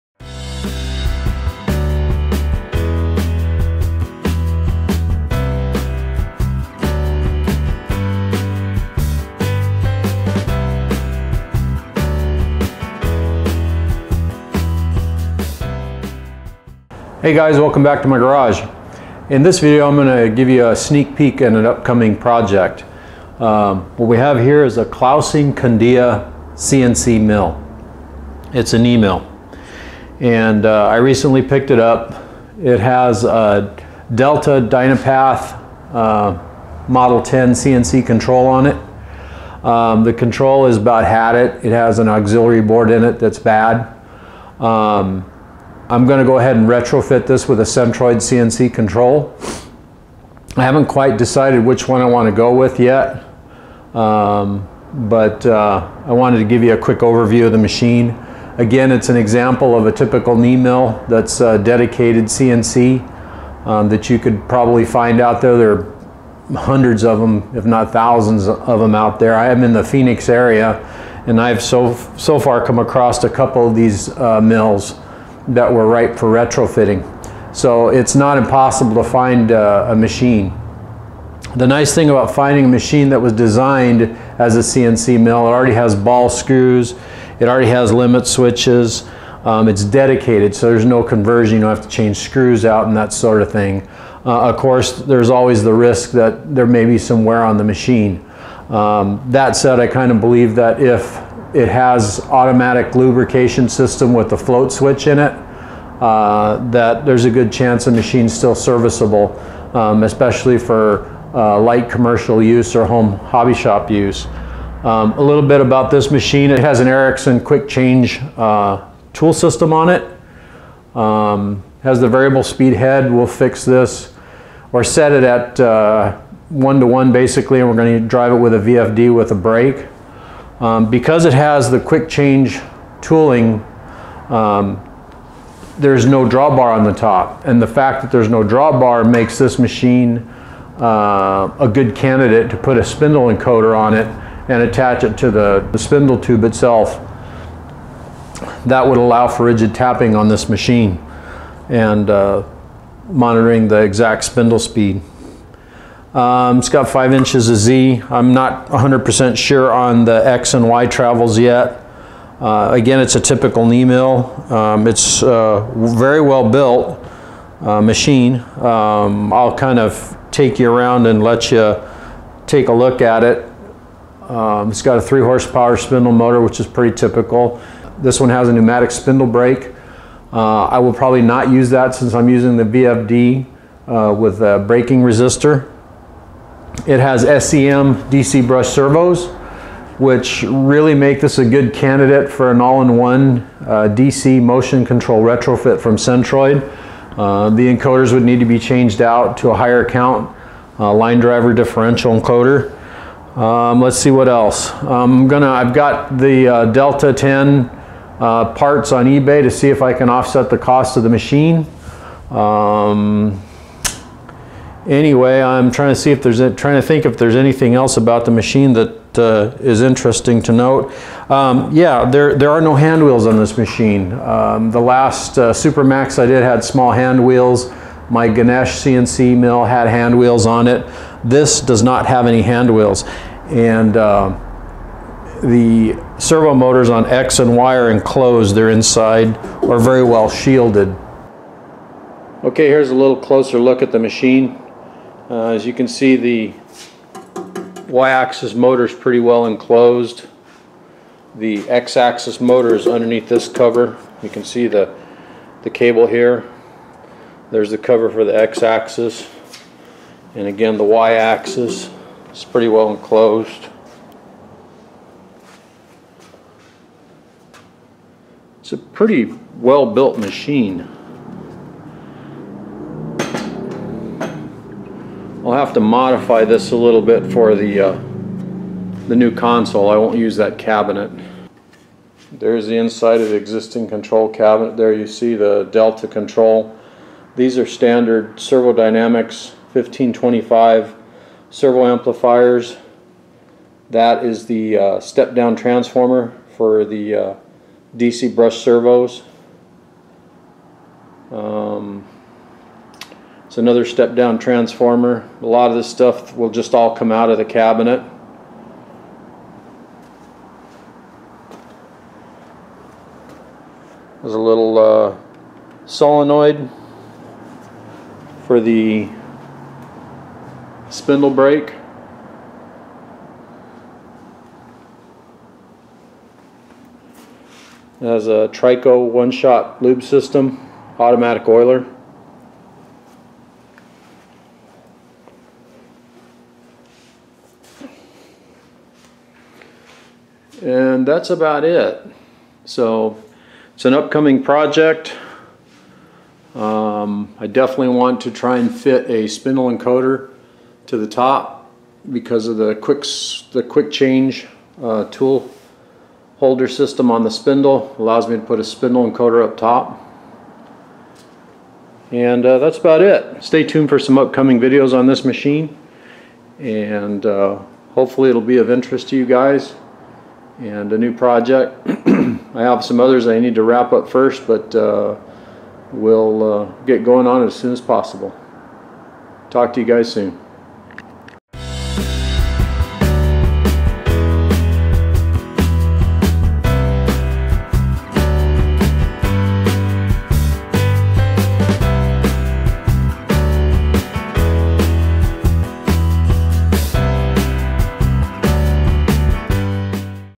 Clausing Kondia CNC Knee Mill Retrofit Overview - Another Centroid CNC Conversion
Yet another new project! I picked up this Clausing Kondia CNC Knee mill with a partially working control.